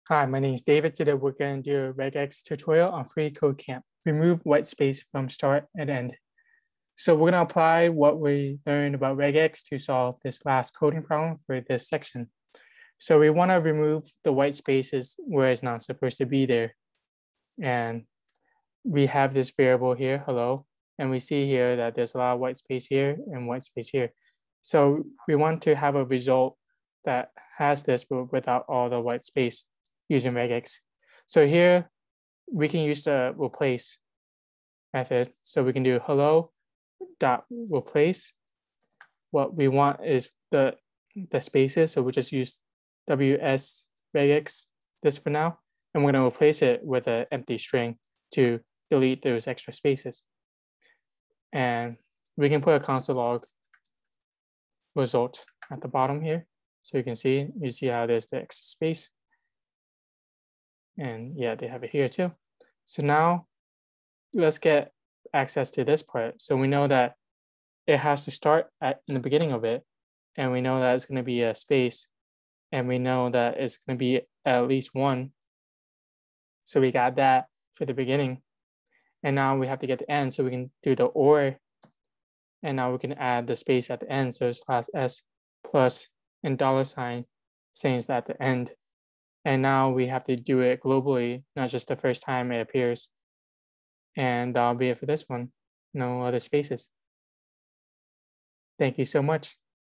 Remove Whitespace from Start and End (Regular Expressions) freeCodeCamp tutorial
Certification: JavaScript Algorithms and Data Structures
Course: Regular Expressions
Lesson: Remove Whitespace from Start and End
freeCodeCamp tutorial

In this lesson, we learn how to remove whitespace from start and end.